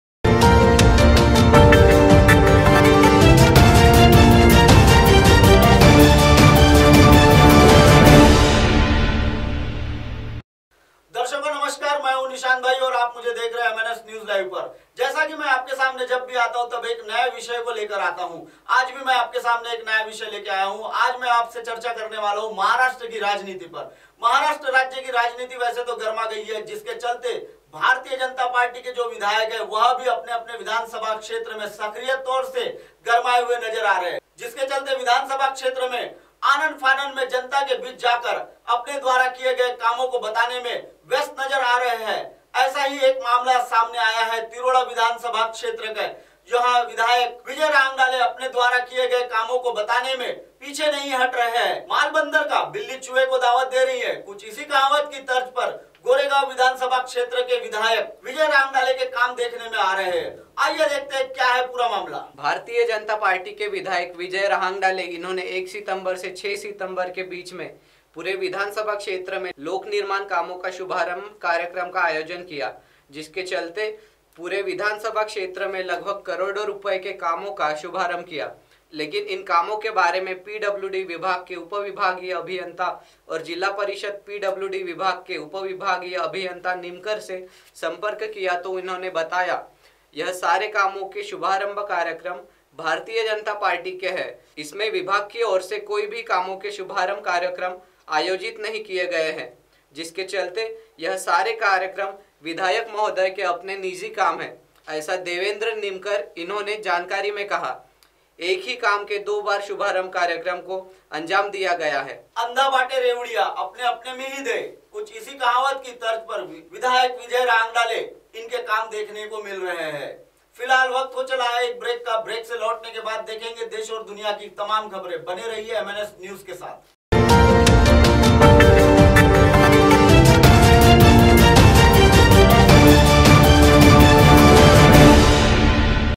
विजय राहंगडाले तिरोड़ा गोरेगाव विधानसभा क्षेत्र के जनता को बना रहे बेवकूफ।
MNSNEWSLIVE विधायक विजय राहंगडाले तिरोड़ा गोरेगाव विधानसभा क्षेत्र के जनता को बना रहे बेवकूफ।